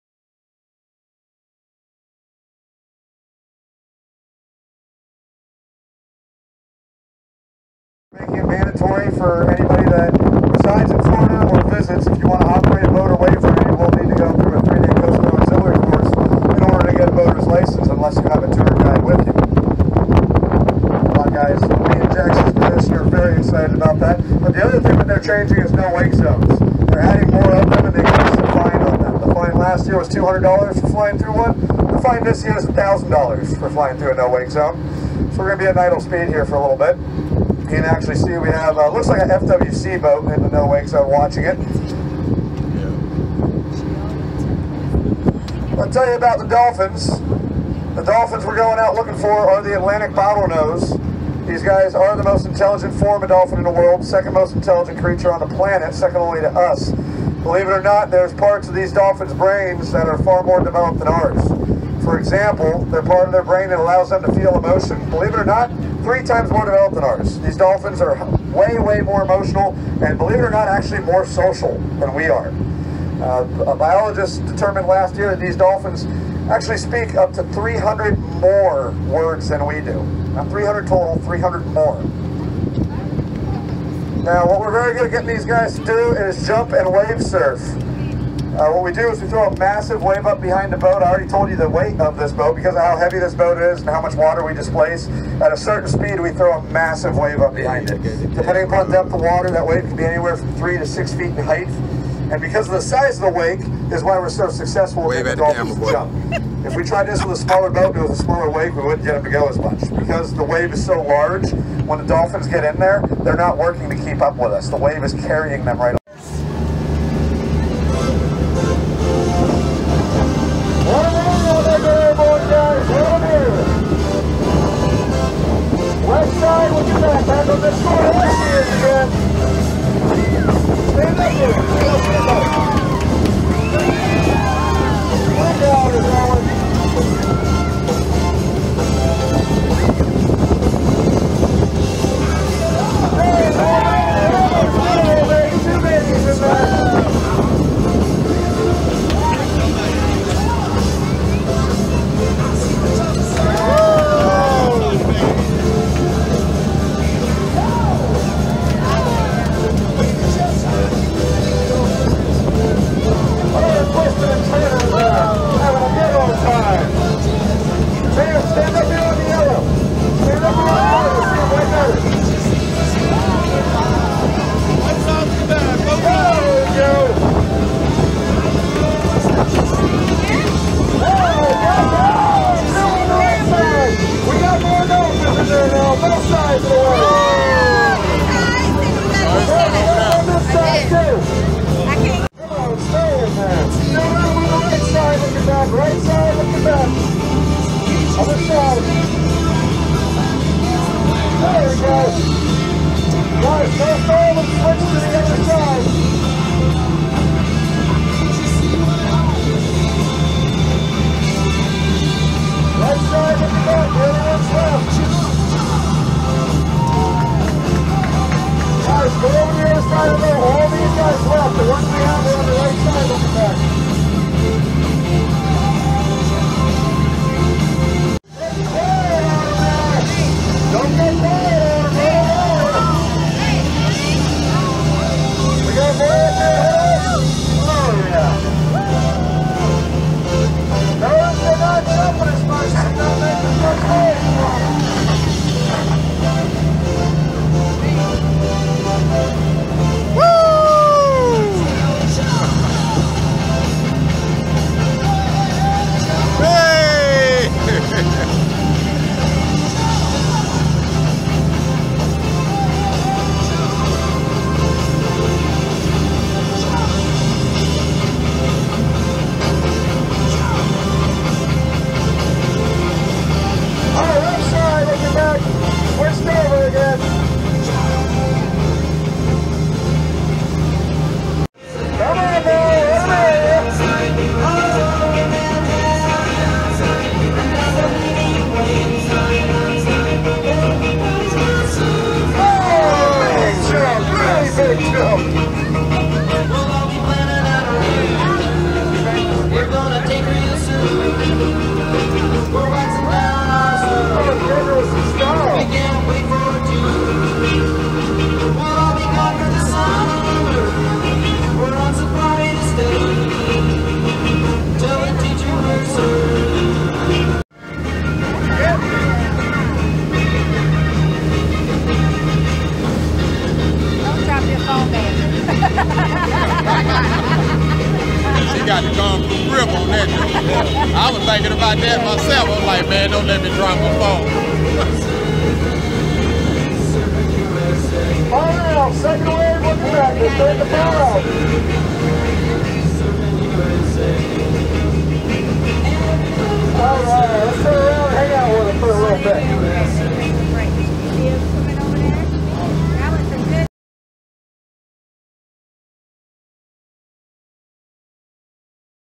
DOLPHIN SURFING SPEEDBOAT ADVENTURE 3400 Pasadena Ave. South, St. Pete Beach
Beautiful day on the water with the family as were enjoying good tunes , the sun, and watching the dolphins entertain us as we speed by on the Dolphin Surfing Speedboat !!